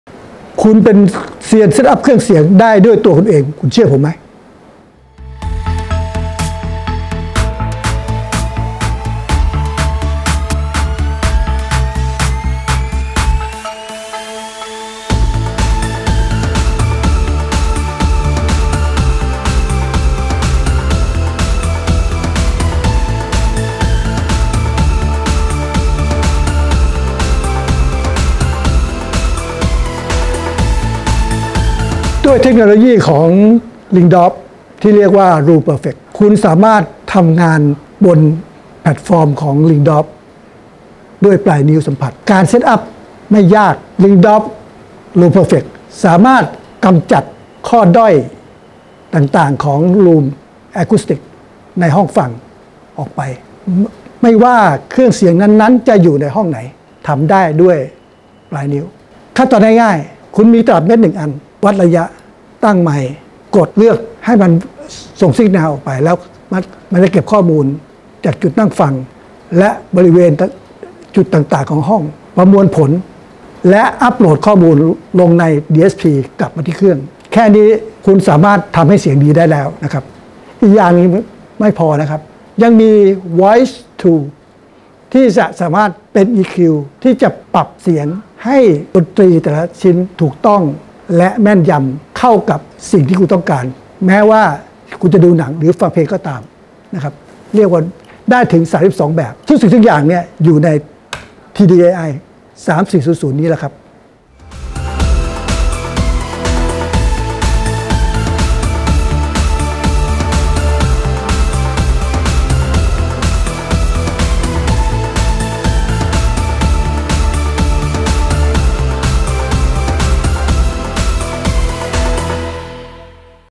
Lyngdorf TDAI 3400... with RoomPerfect & Voicings
คุณเป็นเซียนเซ็ตอัพเครื่องเสียงได้ด้วยตัวคุณเอง ...เชื่อหรือไม่?

ด้วยเทคโนโลยีของ Lyngdorf ที่เรียกว่า RoomPerfect ที่เป็นการปรับระบบเสียงให้เข้ากับสภาพแวดล้อมได้เพียงปลายนิ้วสัมผ้ส ด้วยการกำจัดข้อด้อยของ Room Acoustics ในห้องฟังให้หมดไป ไม่ว่า Lyngdorf TDAI-3400 จะอยู่ในห้องไหน

นอกจากนั้นยังมี Voice Tool ถึง 32 รูปแบบ ที่เป็น EQ ปรับเสียงให้ดนตรีแต่ละชิ้นถูกต้อง แม่นยำ เข้ากับสิ่งที่คุณต้องการ ไม่ว่าจะดูหนังหรือฟังเพลงก็ตาม

ทั้งหมดนี้มีอยู่ใน... Lyngdorf TDAI-3400 (2 Channel True Digital Integrated Amplifier with Audio Processor)

สอบถามรายละเอียดเพิ่มเติม Livingsound โทร.